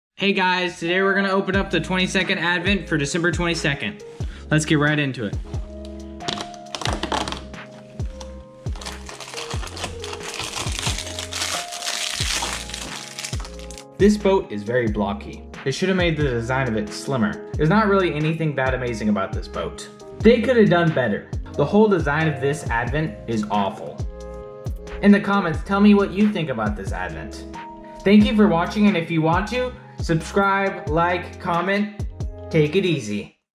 LEGO City Advent Calendar 2020 Twenty-second Advent Boat
Hey Guys , Today were looking at the LEGO City Advent Calendar 2020 Twenty-second Advent Boat.